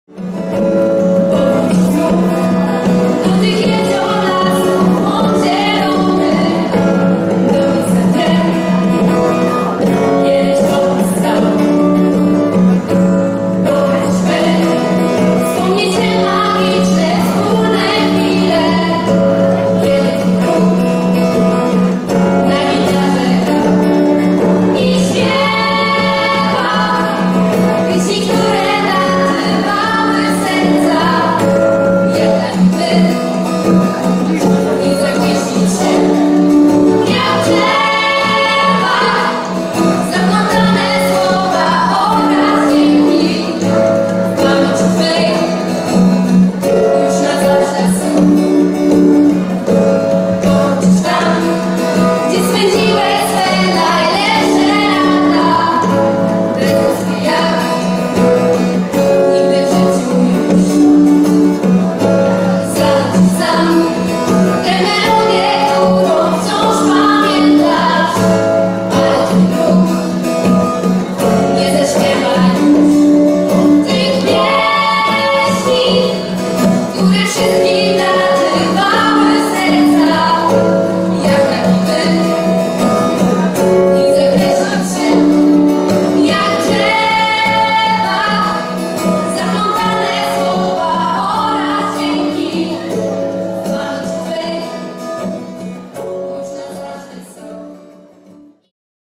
Wspomnienia z 2012 roku hufcowepowroty | Hufiec ZHP Mielec
👉🏻 hufcowepowroty to seria oparta na wspomnieniach. Podkład użyty do filmu pochodzi z występu reprezentacji naszego hufca na Festiwalu Piosenki Harcerskiej i Turystycznej HiT 2012.

hufcowepowroty | Hufiec ZHP Mielec